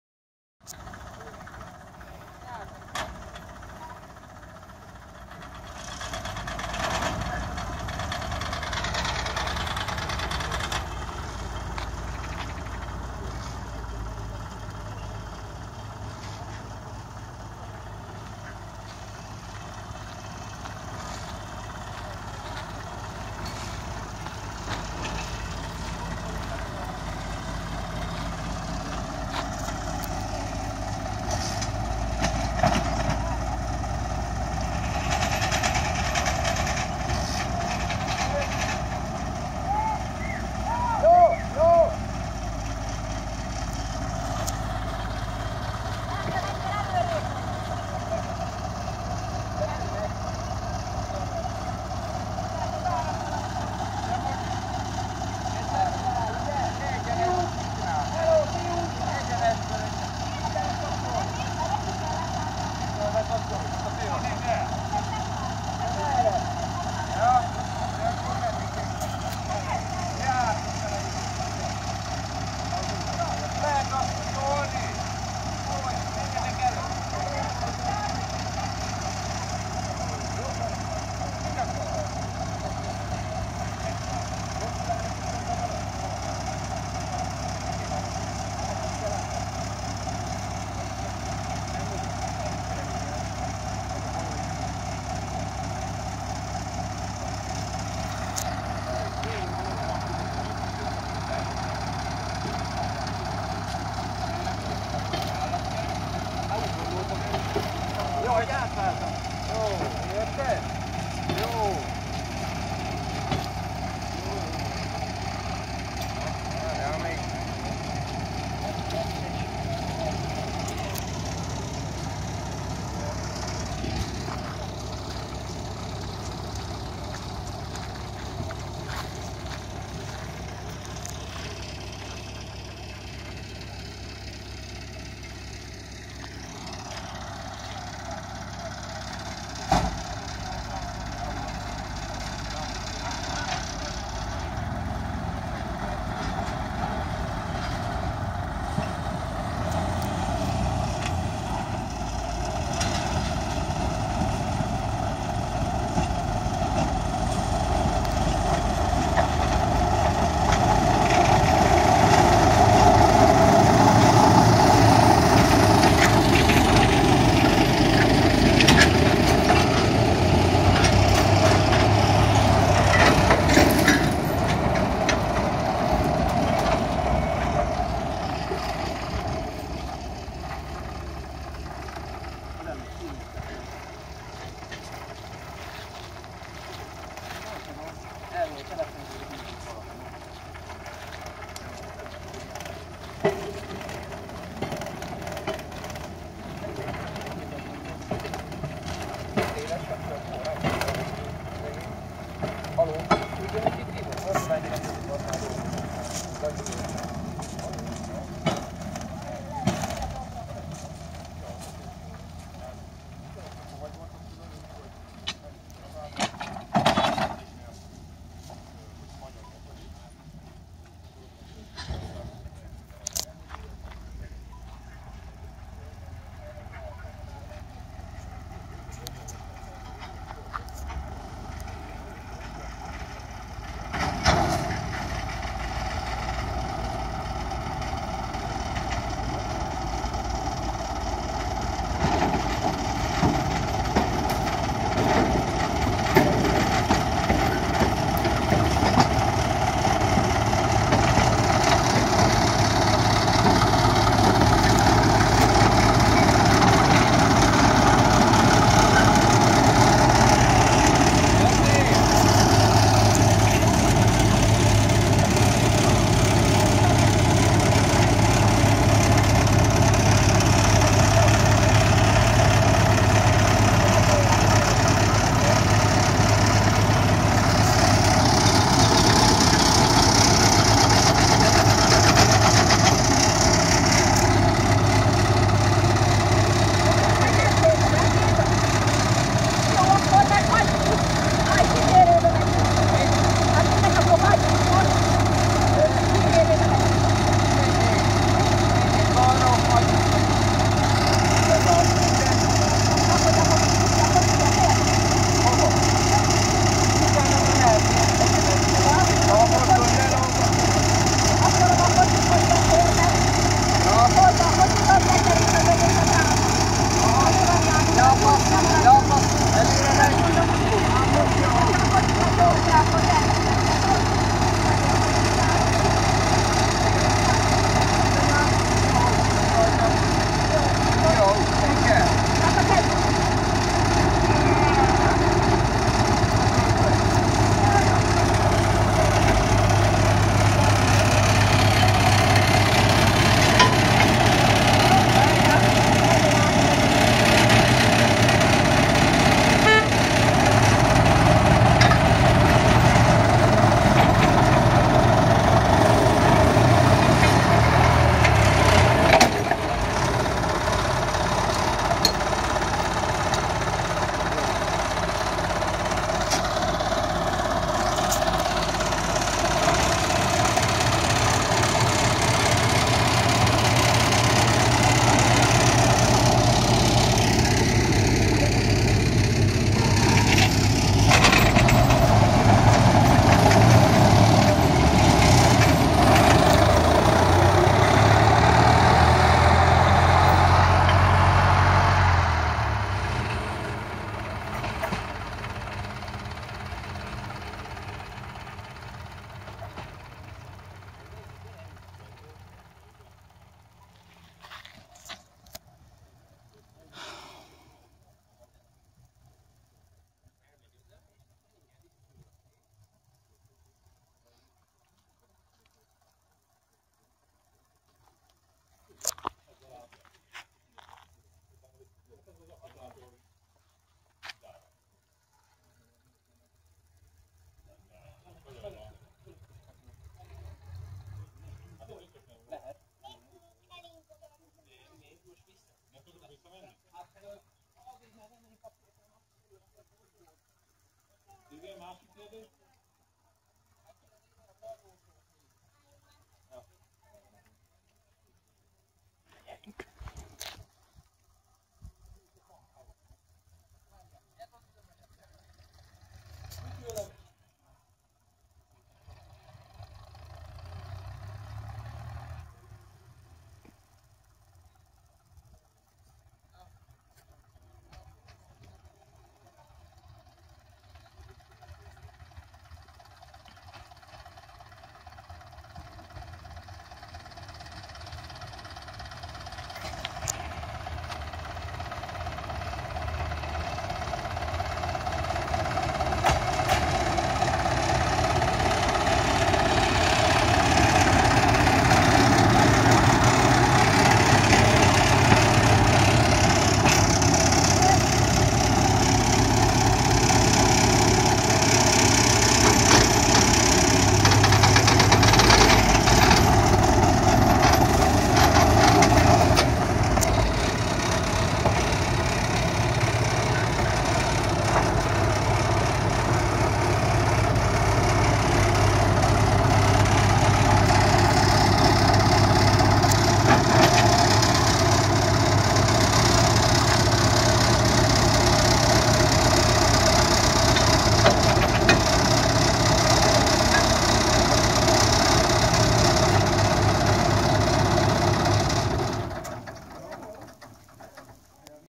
2017.08 Hortobágyi kisvasút tolatási mozgások
Ki kellett szabadítani a vonatot a hajtány mögül.